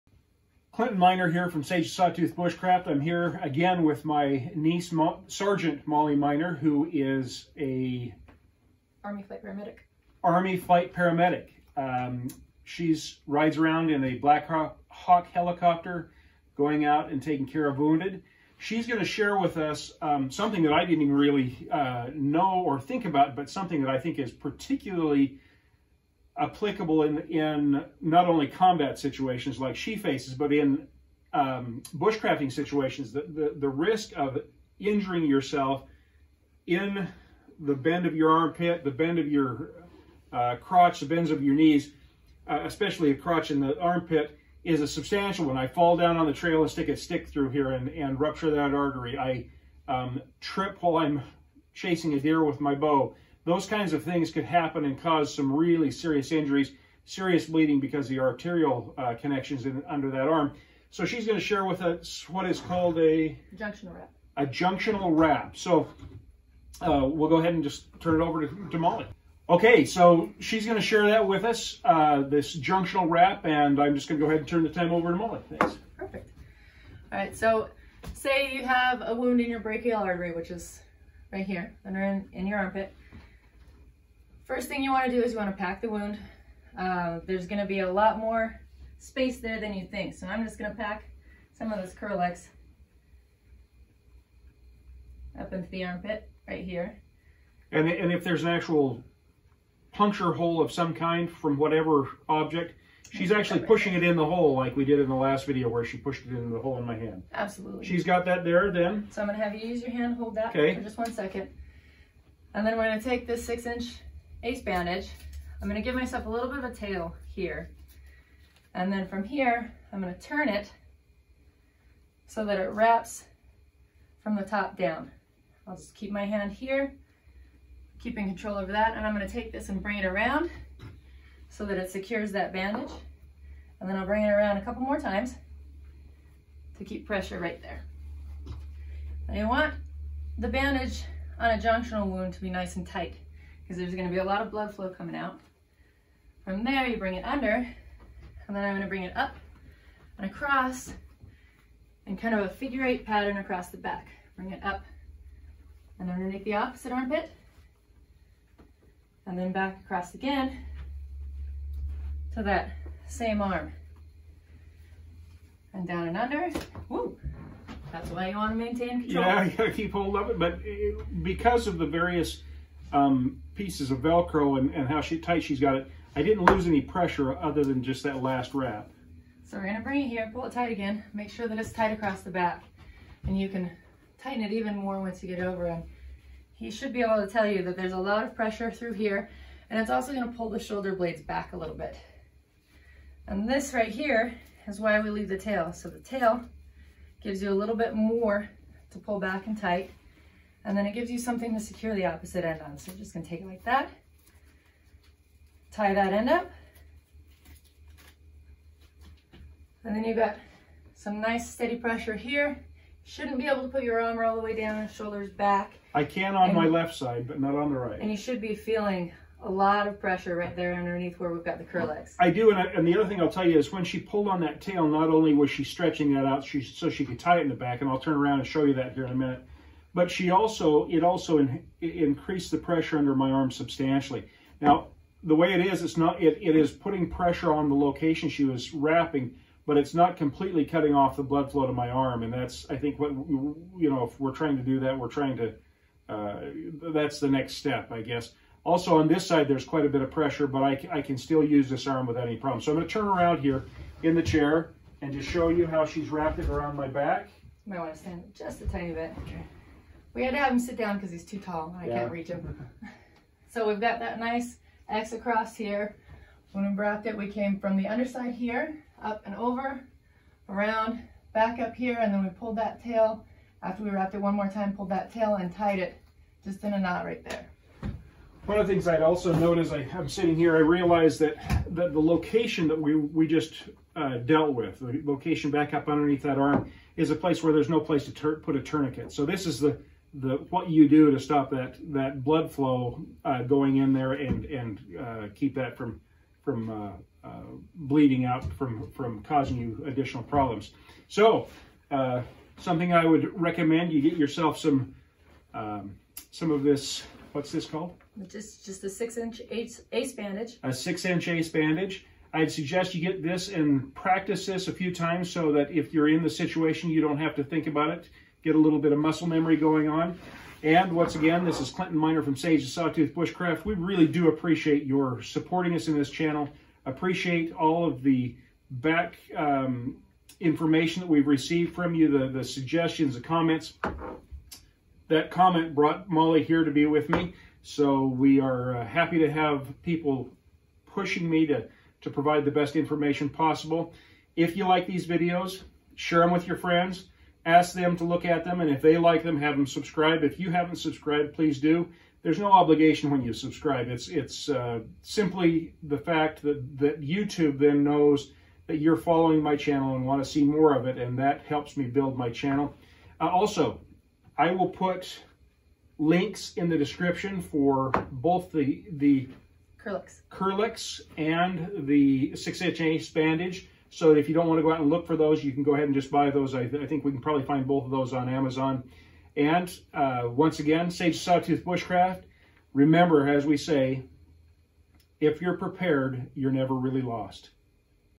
Limb junction use for Compression Bandages
Proper application of a compression bandage at a limb junction. First aid for bleeding at a limb junction such as an underarm (armpit) wound or leg torso injury.

H&H compression bandage
Israeli compression bandage
Kerlix wound packing